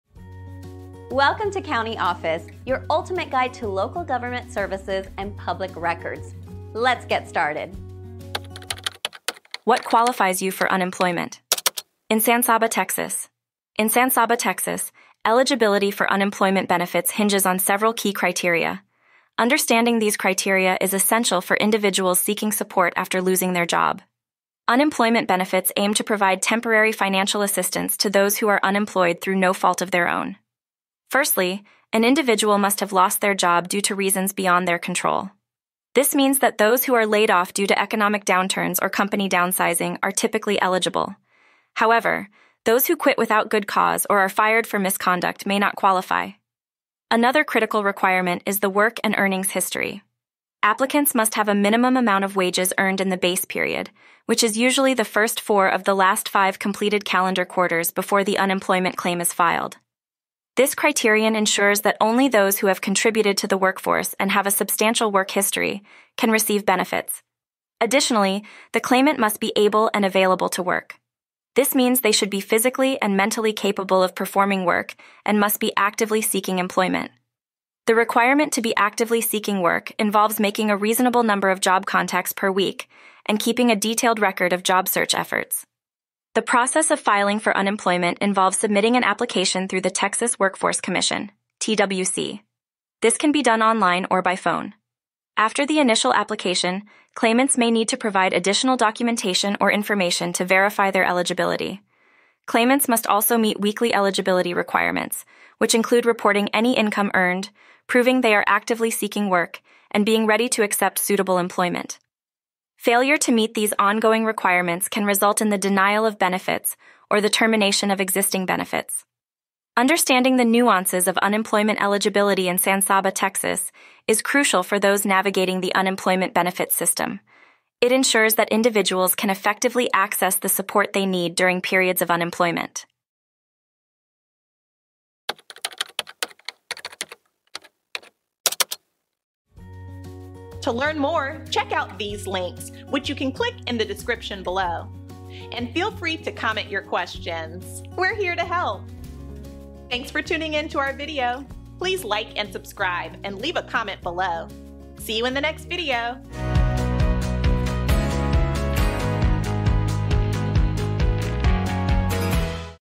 What Qualifies You For Unemployment In San Saba County, Texas? - CountyOffice.org
What Qualifies You For Unemployment In San Saba County, Texas? Navigating the unemployment benefits system in San Saba, Texas, can be a complex process, but understanding the key eligibility criteria is the first step towards accessing the support you need. In this comprehensive video, we delve into the essential qualifications for unemployment benefits in San Saba, Texas. We explore the reasons that may make you eligible for unemployment, such as job loss due to economic downturns, and discuss the importance of your work and earnings history. We also cover the requirements for being able and available to work, including the necessity of actively seeking employment.

Our guide will take you through the application process via the Texas Workforce Commission (TWC), highlighting the importance of providing accurate information and meeting weekly eligibility requirements to maintain your benefits. Whether you're recently unemployed or seeking to understand the unemployment benefits system better, this video offers valuable insights to help you navigate the process.